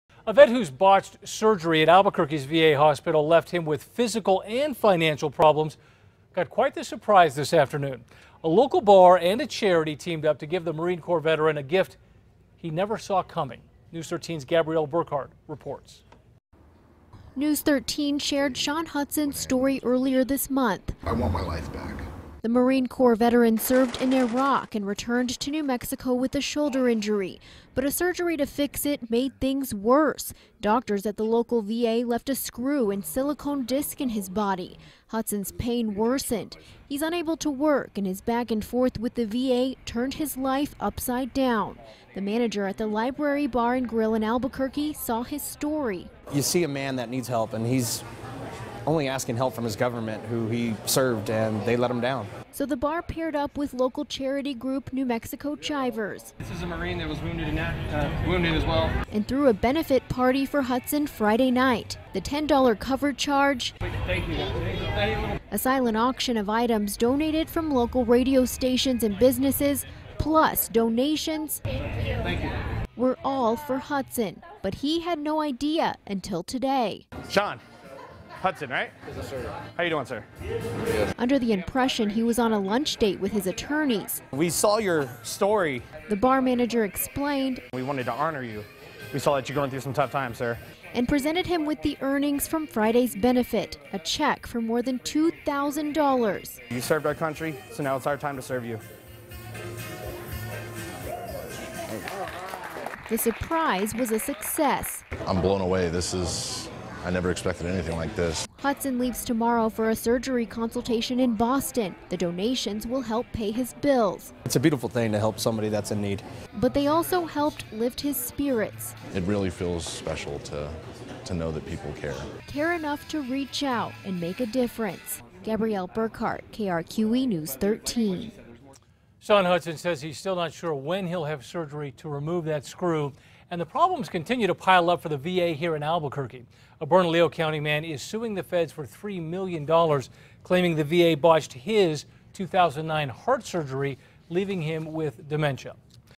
Local group surprises struggling NM Veteran
Bar manager teams with charity to help local veteran.